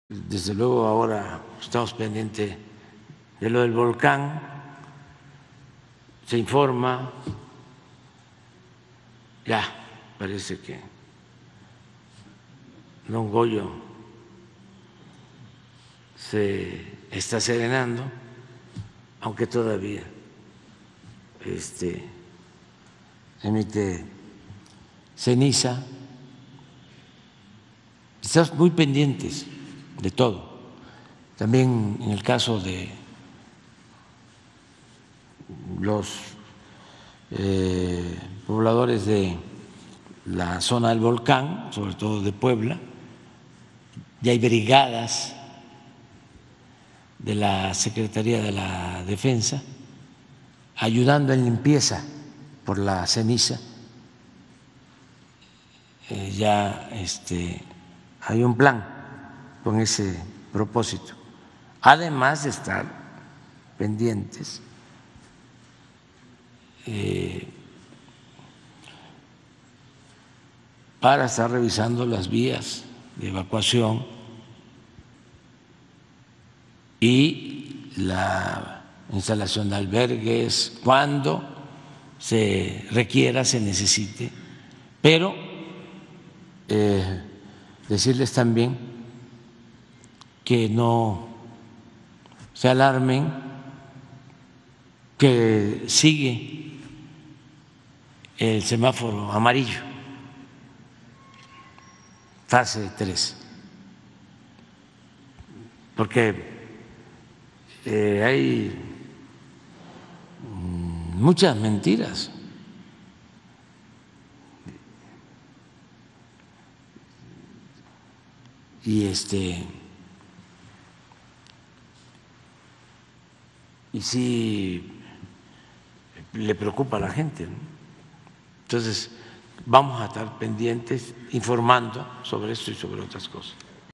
Parece que Don Goyo se está serenando, aunque emite ceniza: AMLO; resaltó que Sedena está alerta
"Parece que Don Goyo se está serenando, aunque emite ceniza", dijo López Obrador; resaltó que Sedena se encuentra en la zona y pide a la ciudadanía no alarmarse